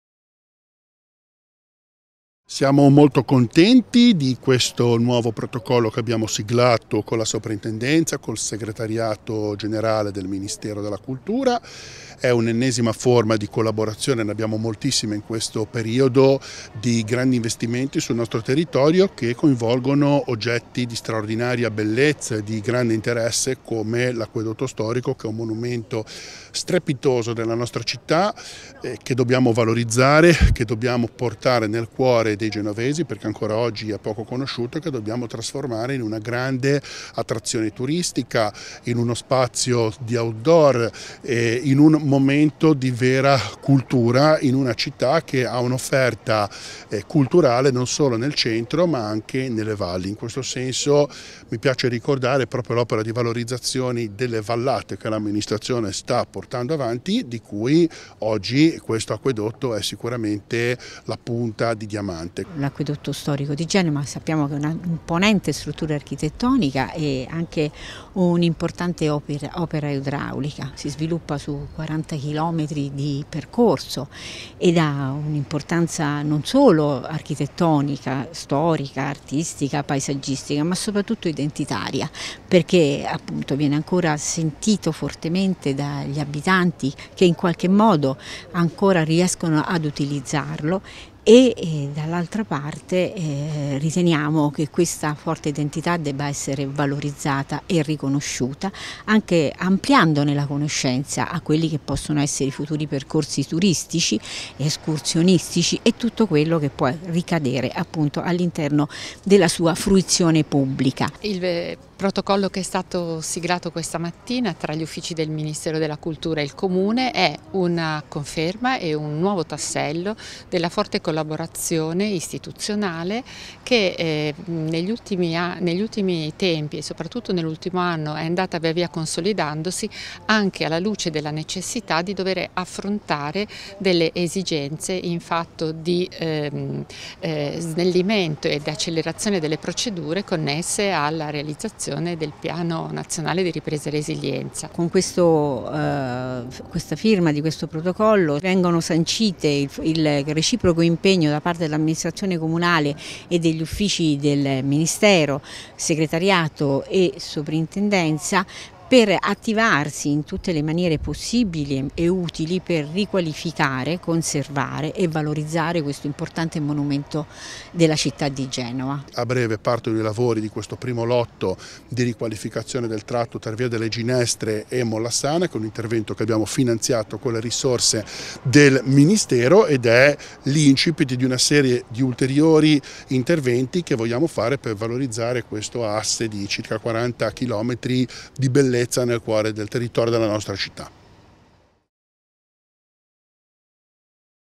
Protocollo d'intesa per l'Acquedotto Storico
Firma del Protocollo d'intesa per la riqualificazione dell'Acquedotto Storico
Genova, 26 ottobre 2022

riprese e montaggio di Alessio Bixio
2022©Centro Video Comune di Genova